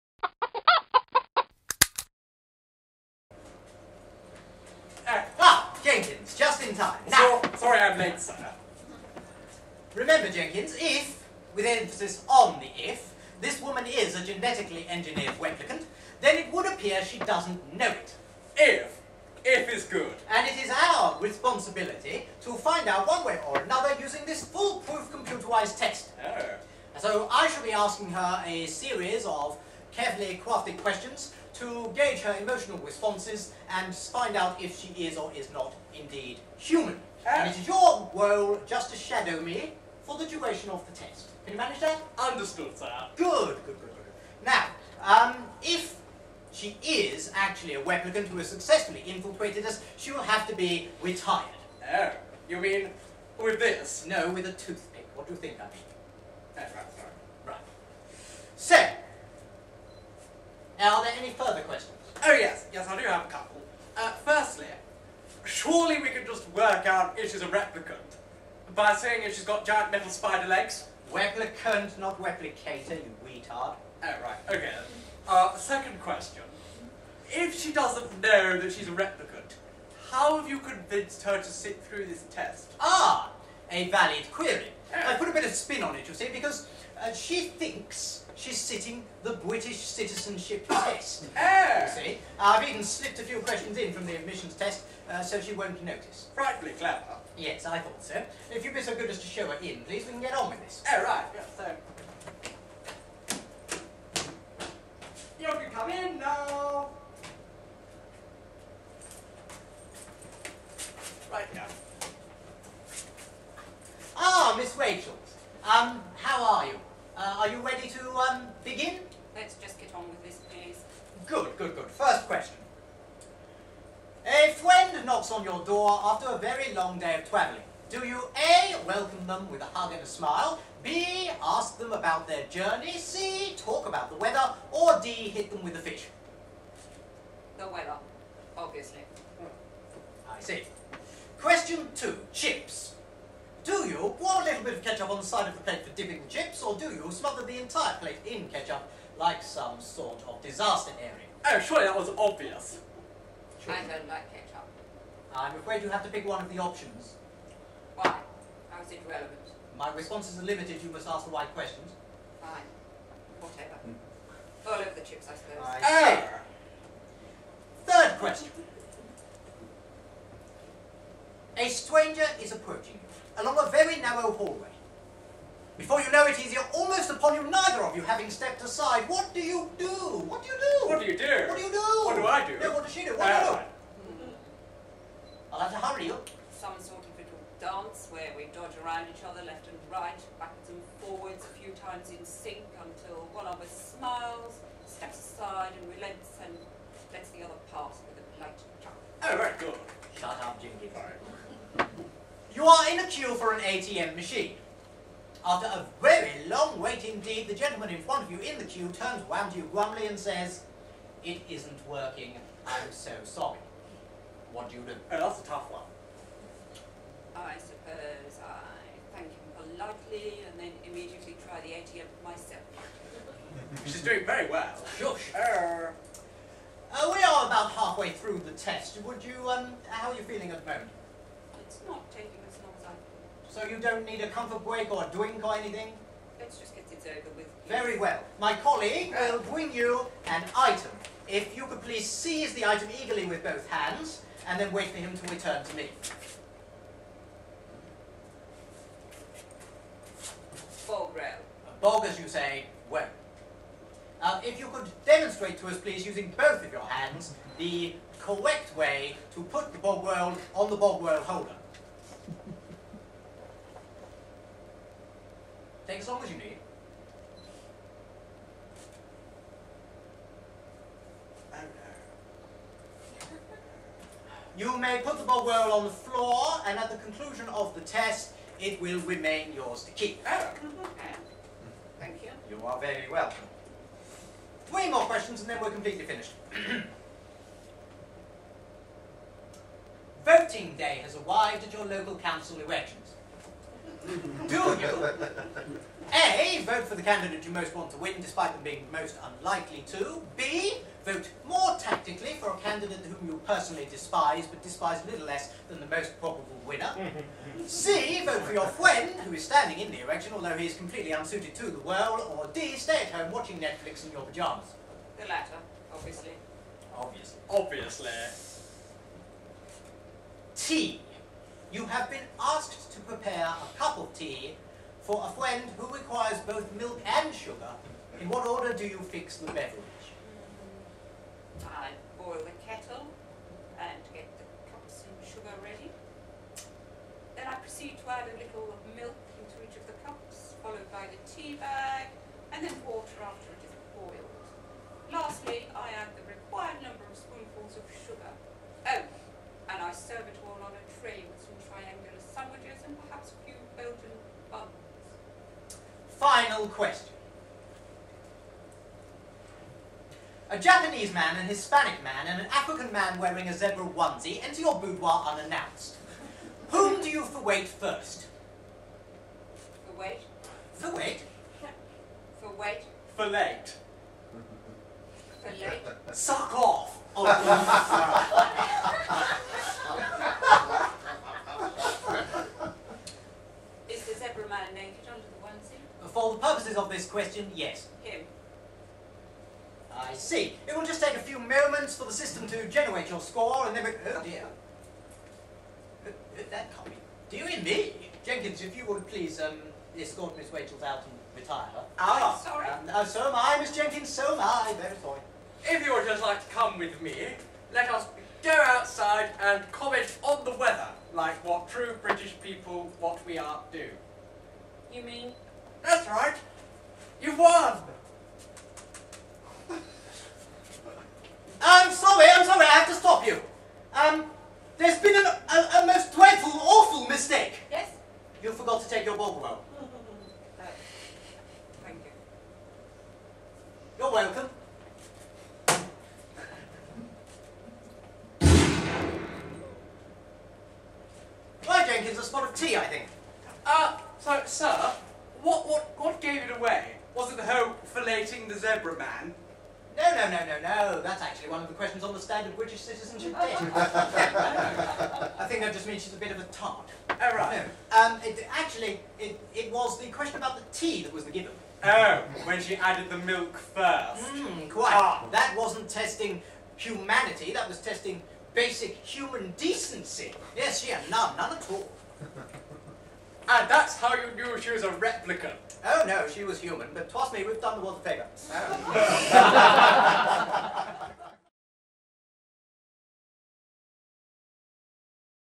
Question Time
In order to confirm her status as a human being one woman must undergo a harsh test, with her life hinging on the answers she provides.

A comedy parody sketch by Stapled to a Chicken. Taken from their 2017 summer show "What Planet Are You On?". Written by Matt “Bob” Southgate, featuring Keith Hiscox, Sue Southgate, and Jamie Gordon.